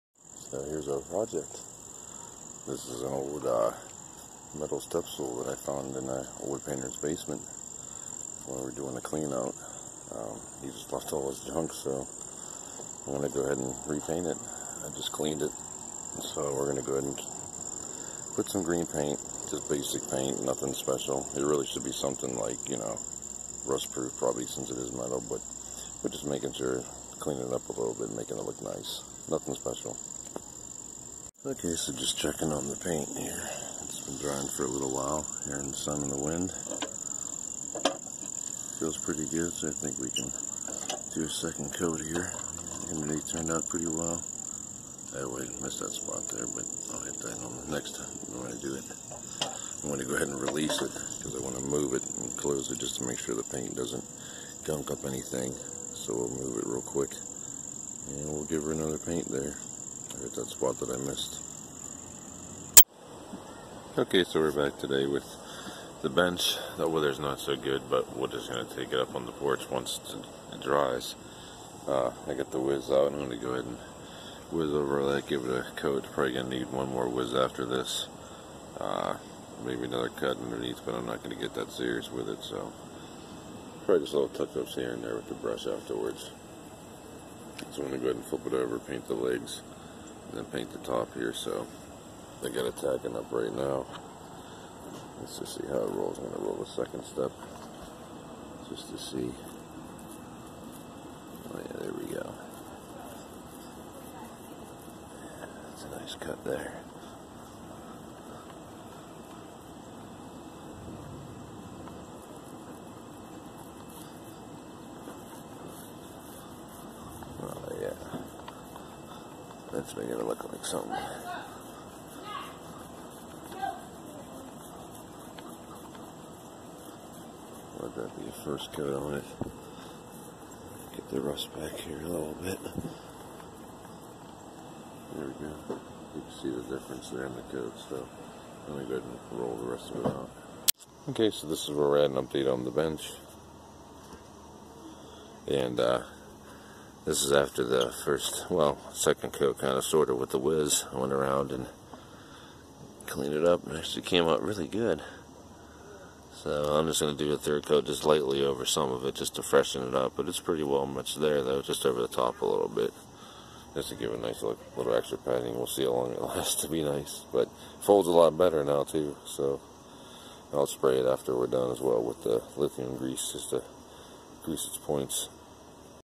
Metal Step Stool Restore
Cleaning up and repainting an old metal folding step stool, found it in an old house we remodeled some time ago. Looked like an old painter owned the house had all kinds of all painting equipment in the basement.